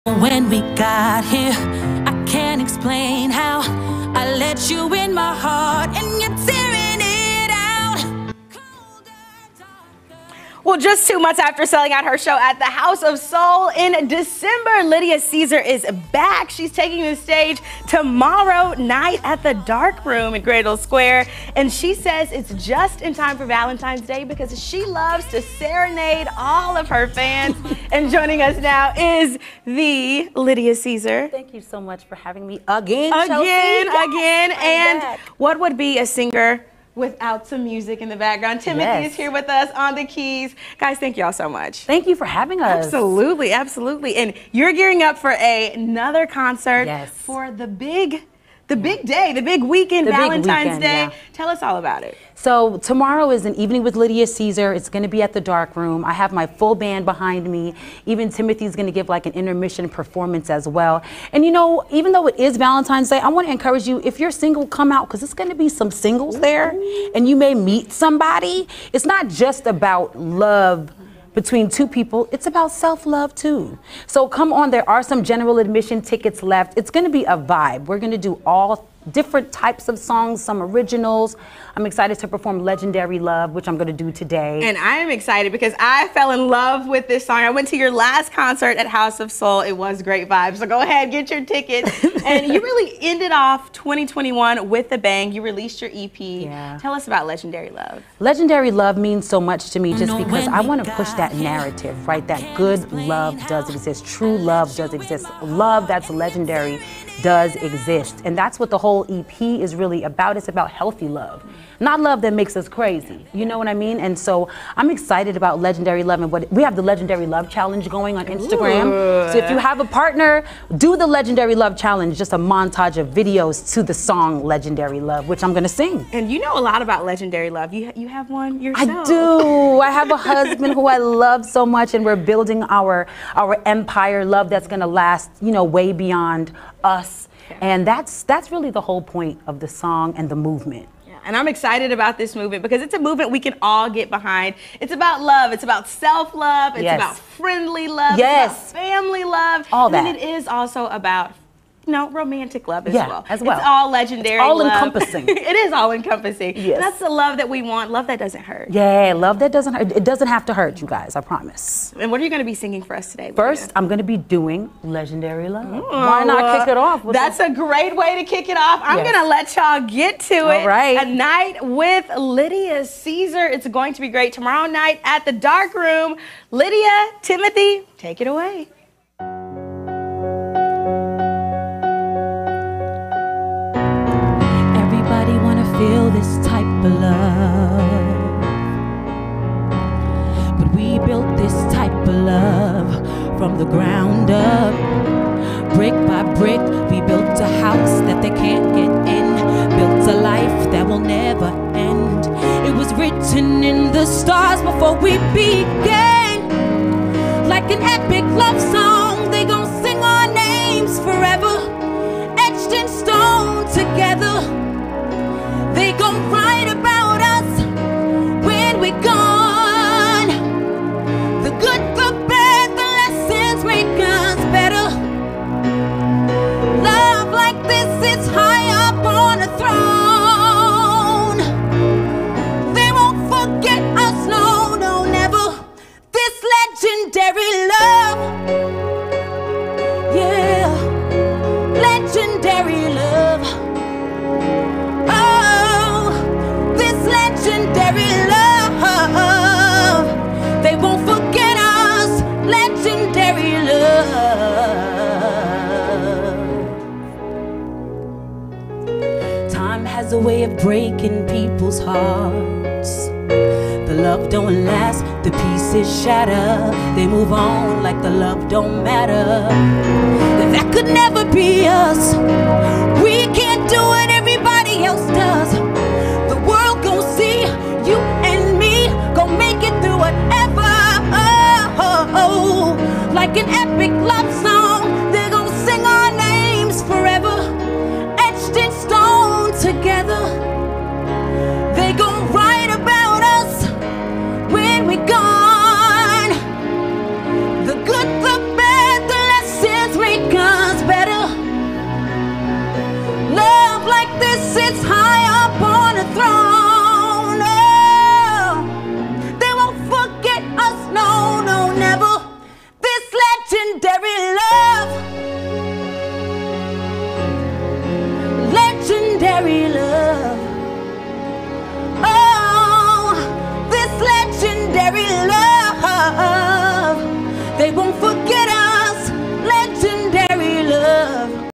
Love is legendary with rising star Lydia Caesar
Lydia Caesar is starting off the year in love! She performs live on Studio STL ahead of her show this weekend.